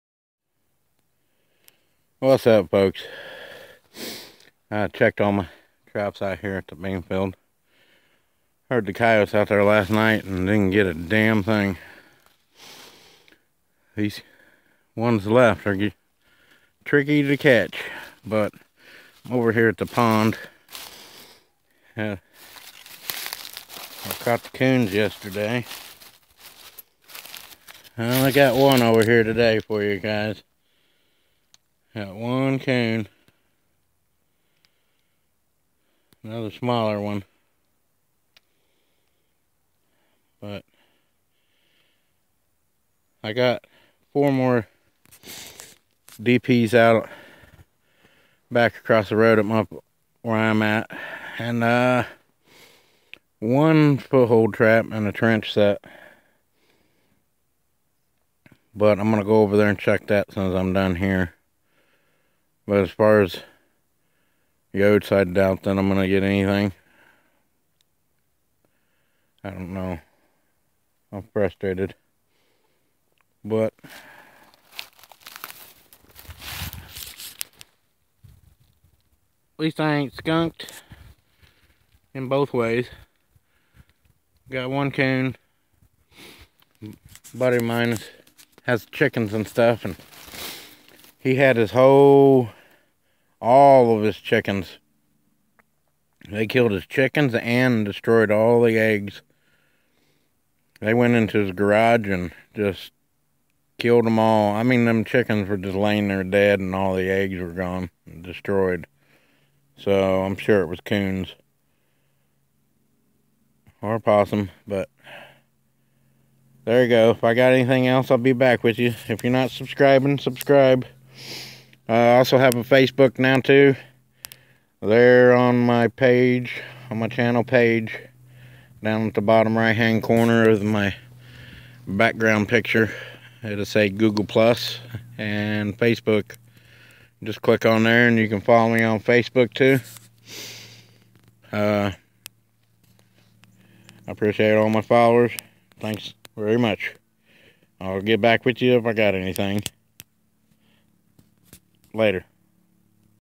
One racoon at the pond.missouri trapping.
Just one coon today folks.